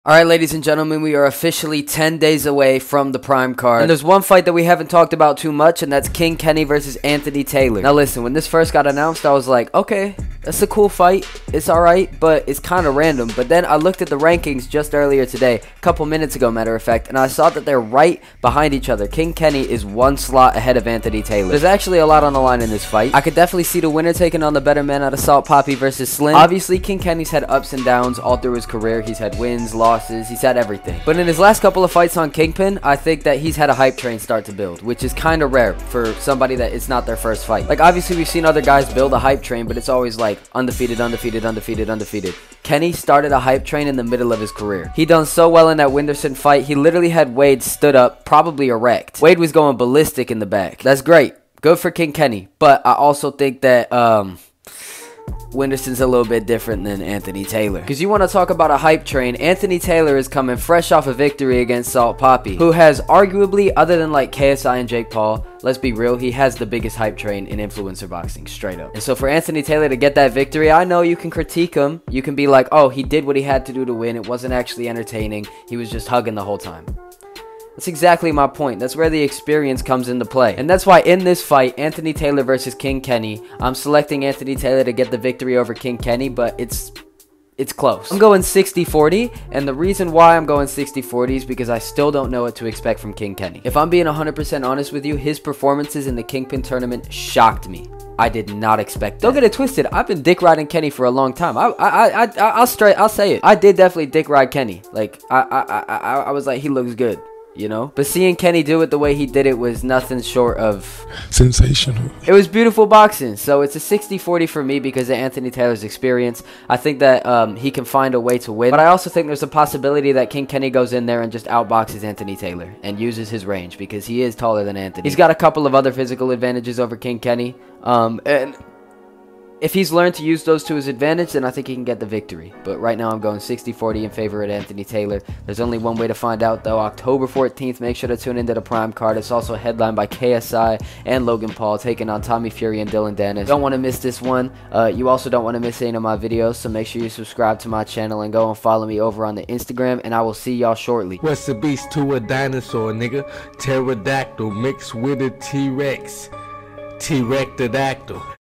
The TRUTH About King Kenny Vs Anthony Taylor…
King Kenny vs Anthony Taylor is going down on the KSI vs Tommy Fury card, and here are my honest predictions.

The Prime Card is also stacked up with Salt Papi vs Slim, Deen The Great vs Walid Sharks 2, and many other great matches.